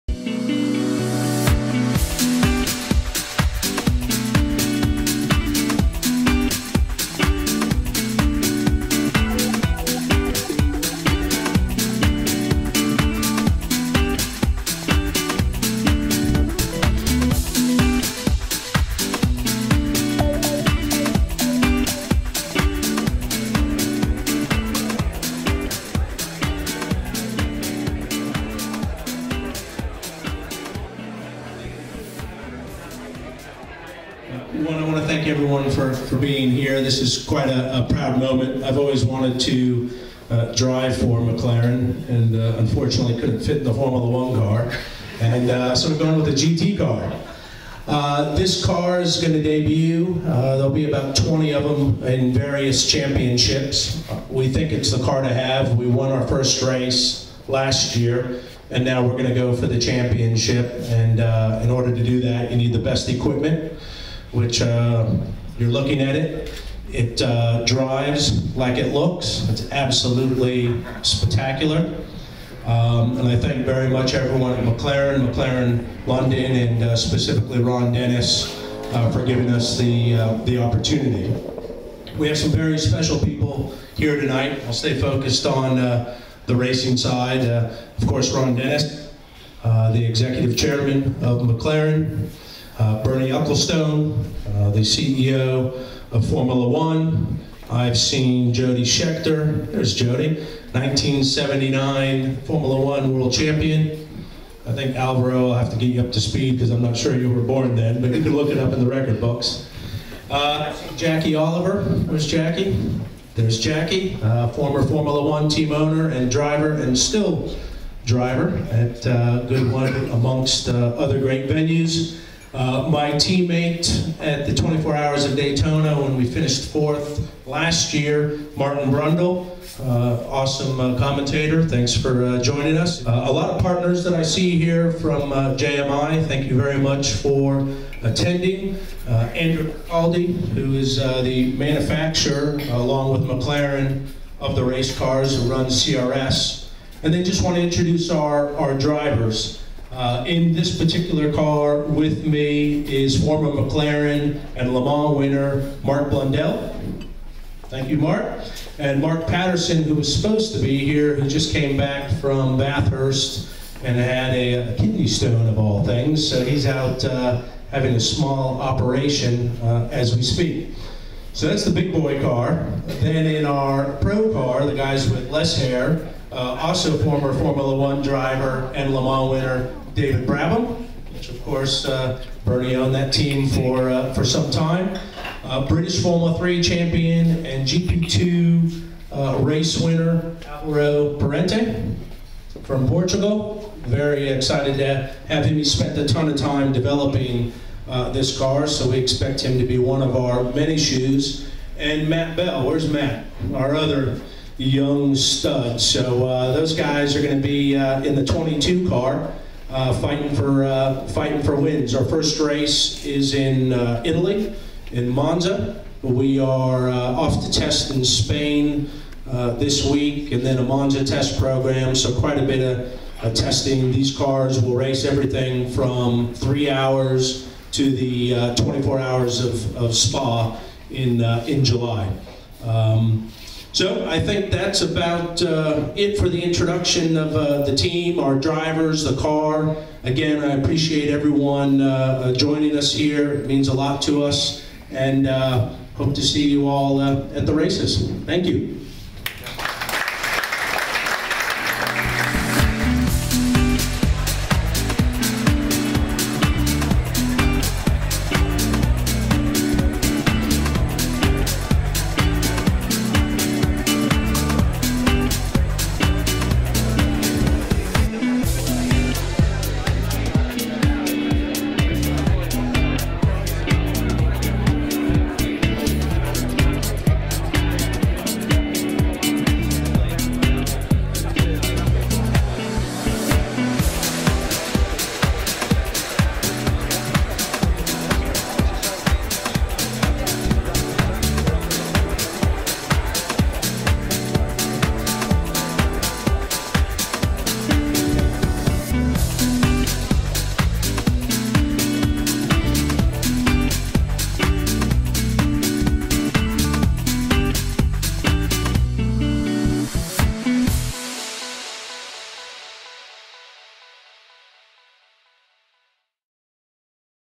United Autosports' McLaren Launch
United Autosports launch their McLaren MP4-12C GT3 racing campaign at McLaren's showroom in Knightsbridge, London.